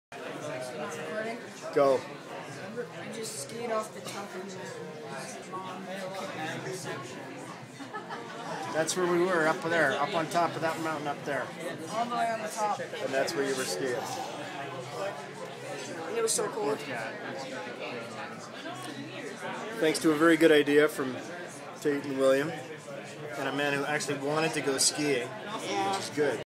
Lunch at Chimney Corner (Sunshine Village)
Great day on the slopes. This was lunch time.